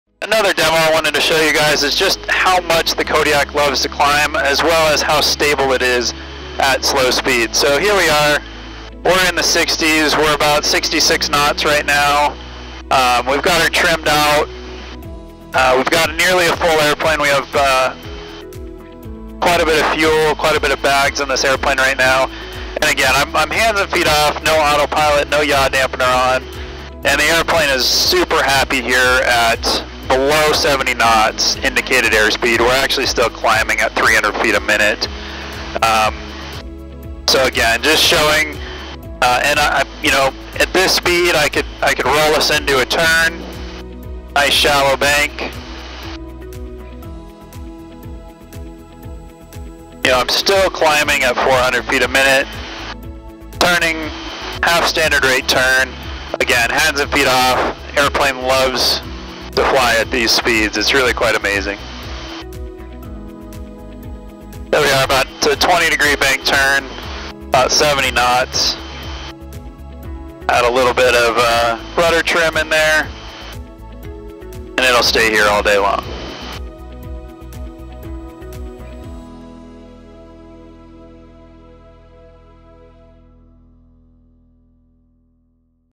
Stability Demo - Kodiak 100
The stability of the Kodiak 100 is unparalleled in an aircraft in this class. Watch as our Chief Demo Pilot, Mark Brown, demonstrates the ability of the Kodiak to loiter and turn at 68 KIAS with minimal inputs. This stability is vital in critical phases of flight.

Our Special Missions operators rely on this stable platform for "head outside time" - time spent looking at their mission rather than worrying about the aircraft. When the airplane does exactly as the pilot commands, it increases awareness all around the cockpit.